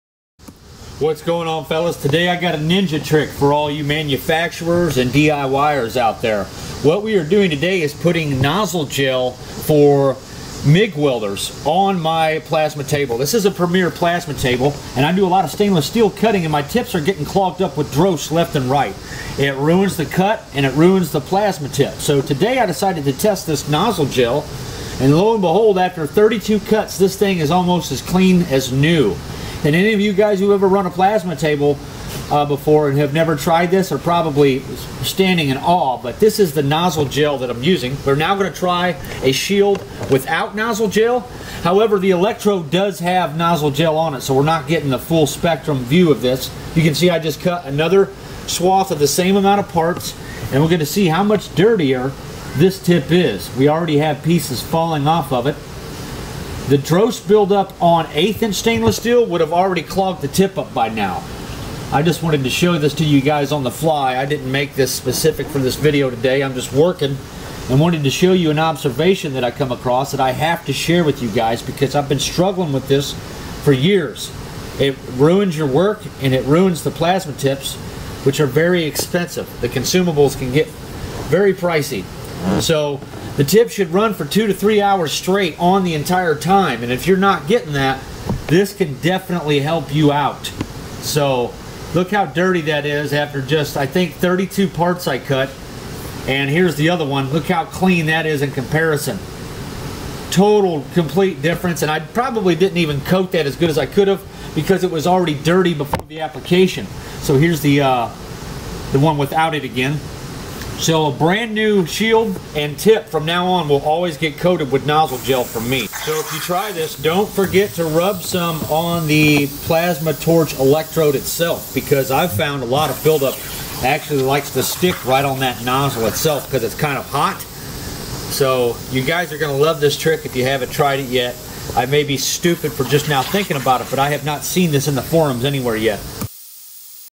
Stop dross Build up in plasma table torch tips SOLVED
Plasma Torch shields getting clogged on your plasma table ?
My premier plasma table has bin running strong for 3 years but 1 problem i just now solved was my shields and tip clogging up with dross build up .
When Dross build up starts to effect torch height the the electrode life ends fast . Also the cut quality is reduce because of torch tip dross build up .